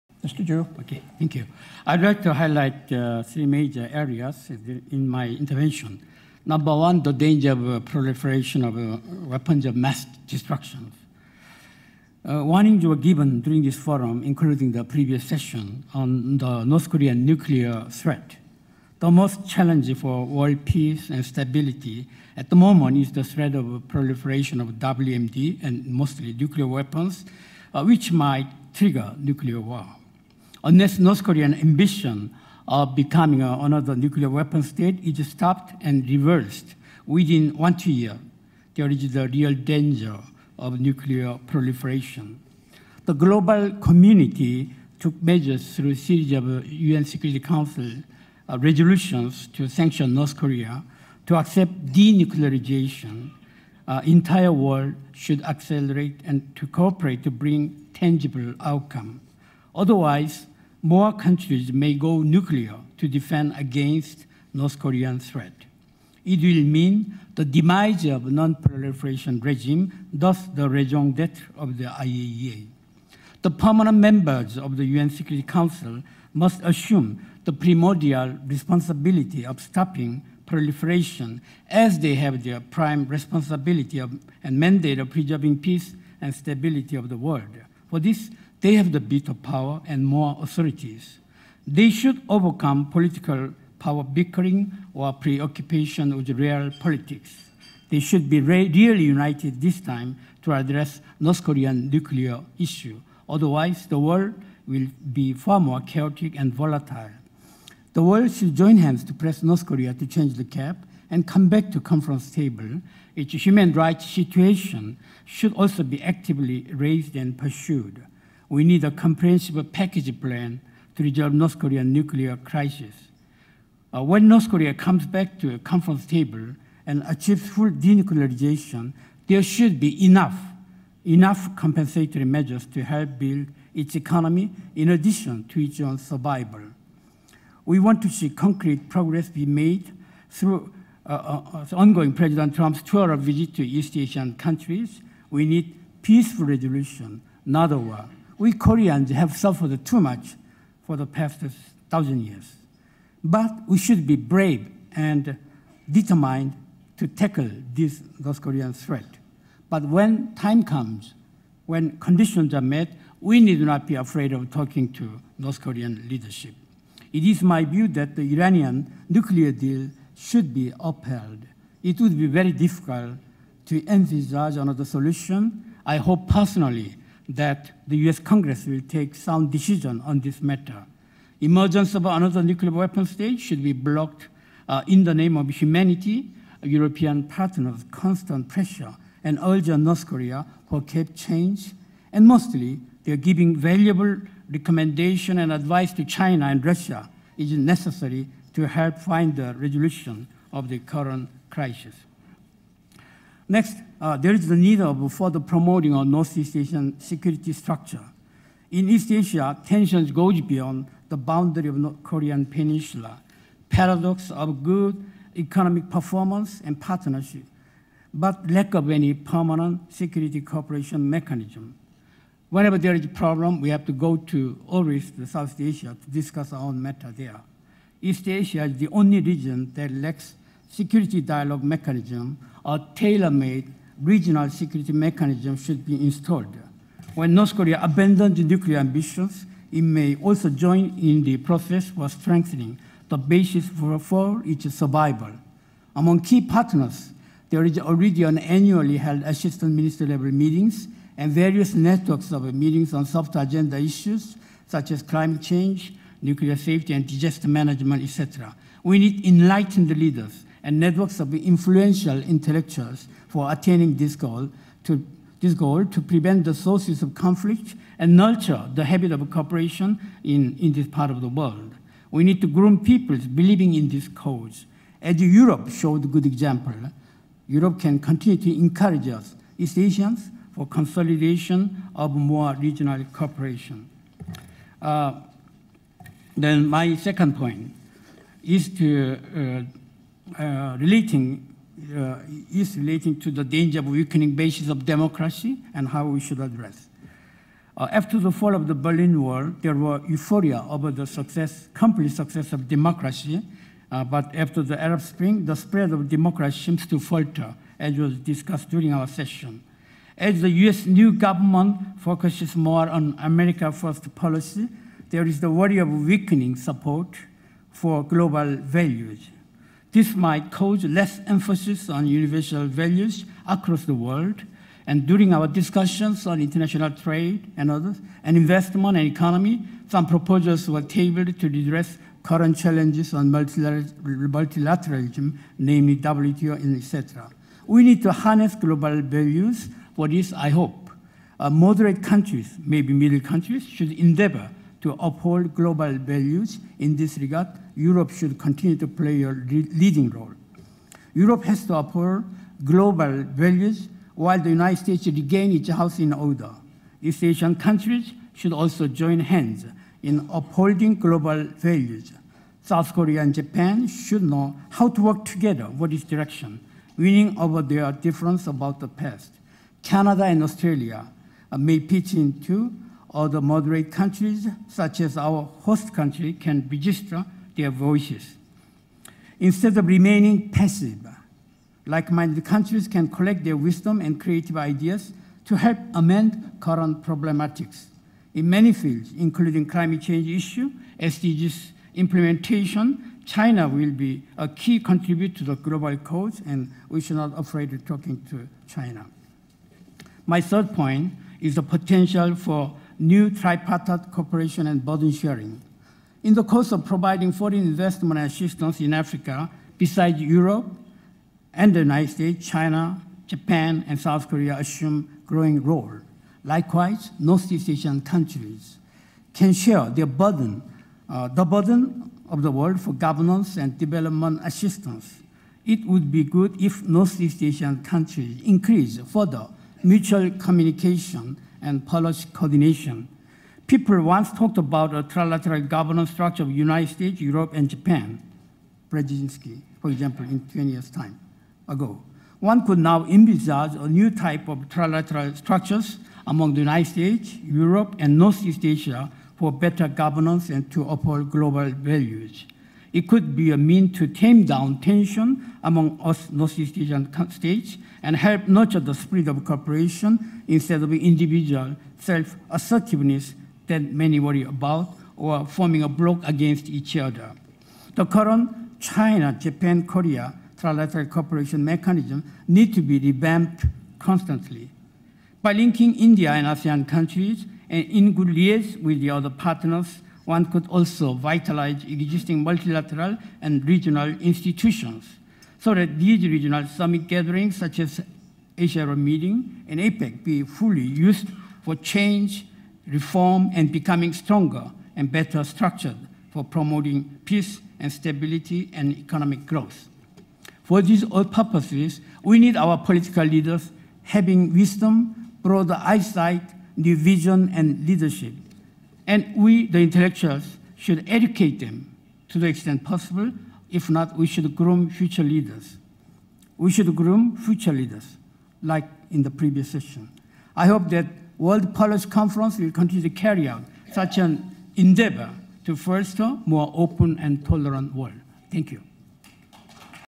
WPC2017 S16 - Ju Chul Ki EN
Plenary session 16: The state of the world
Ju Chul Ki, President of the Overseas Koreans Foundation, Former Senior Secretary for Foreign Affairs and National Security, Office of the President of the Republic of Korea